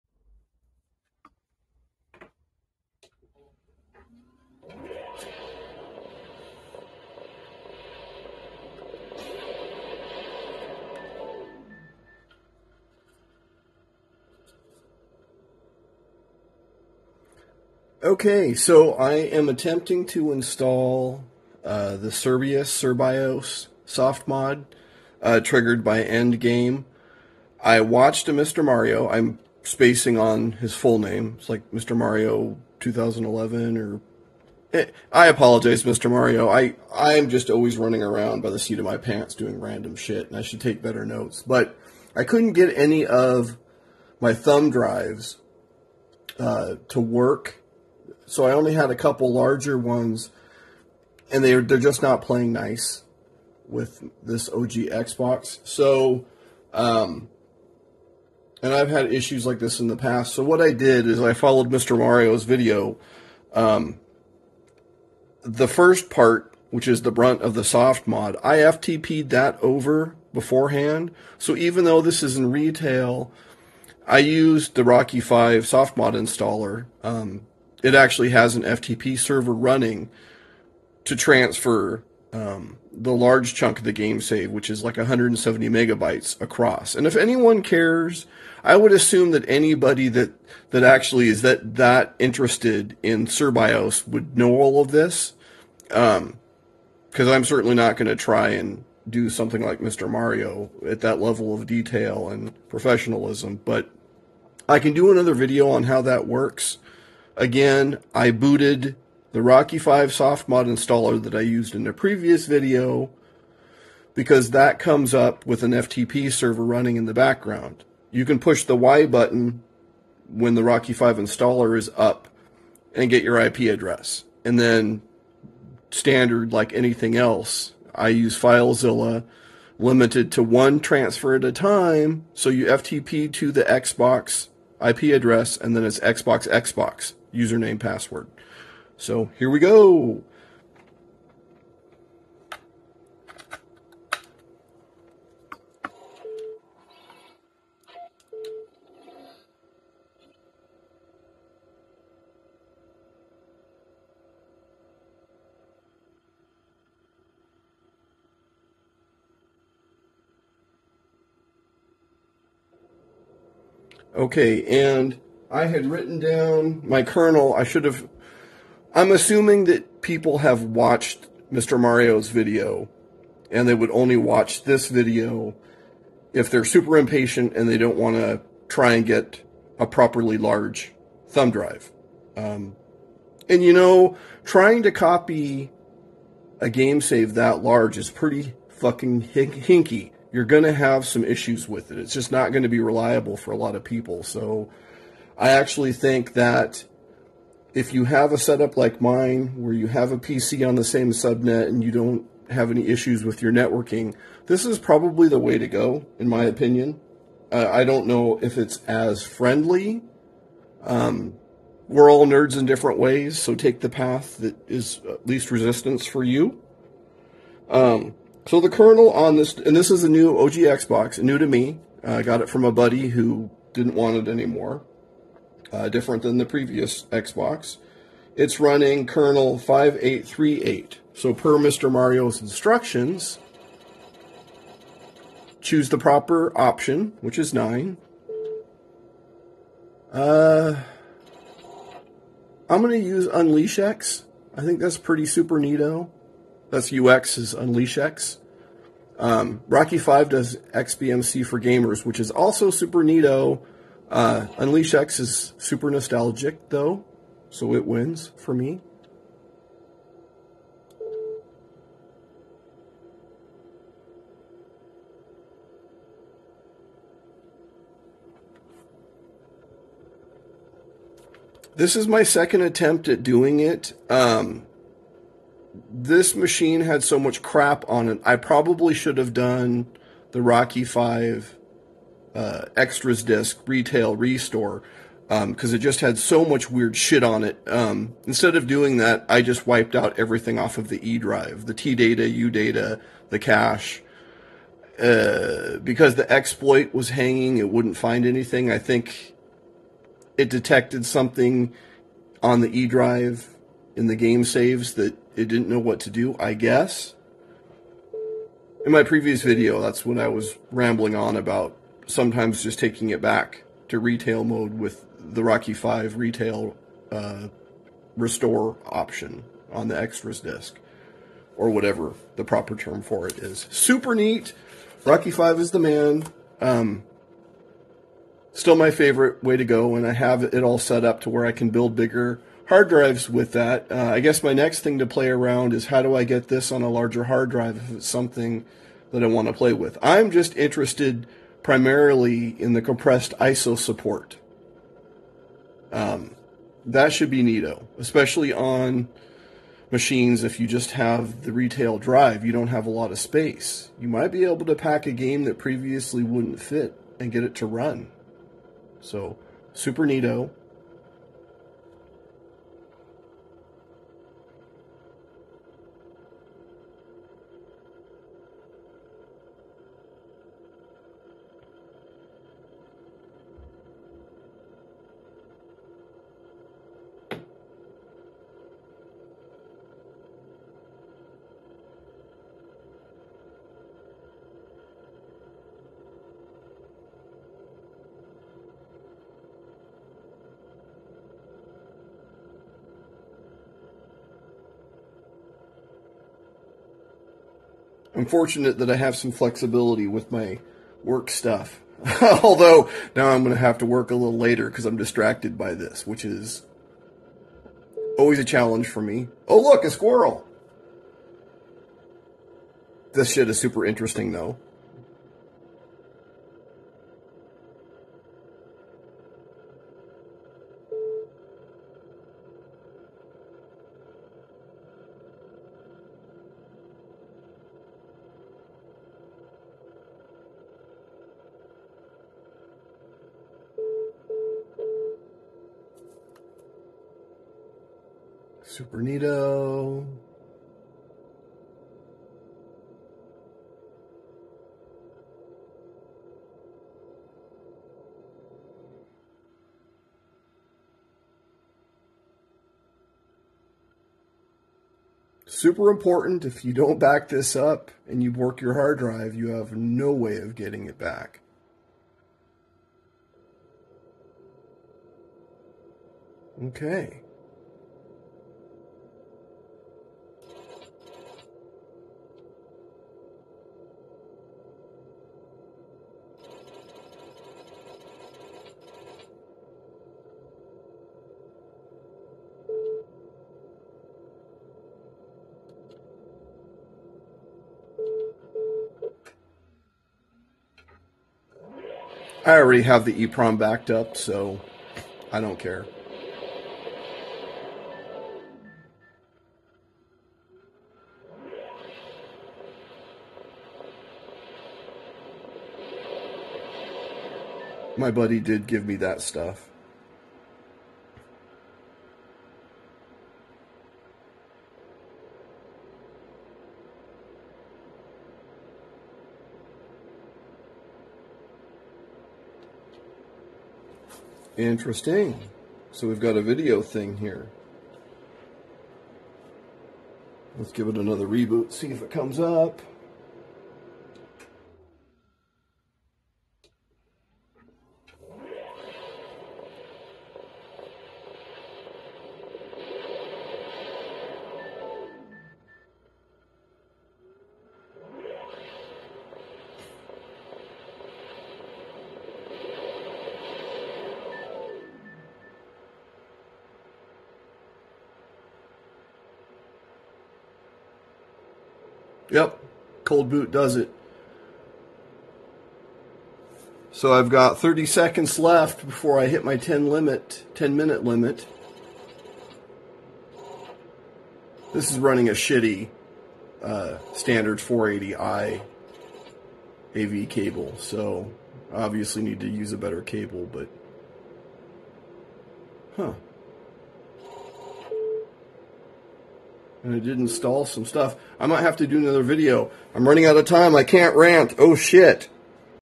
Cerbios BFM USB Softmod Installer using standard Xbox Memory Unit.
I used this: OGXBox Cerbios BFM USB Softmod Installer v2.3.1 Build 07 ENDGAME (09 Mar 2024).rar

I used the FTP server capability of Rocky5's softmod installer to copy \Softmodding Pack\21585554 to E:\UDATA - this replaces the need to have a thumb drive capable of fitting the 170MB of modding files.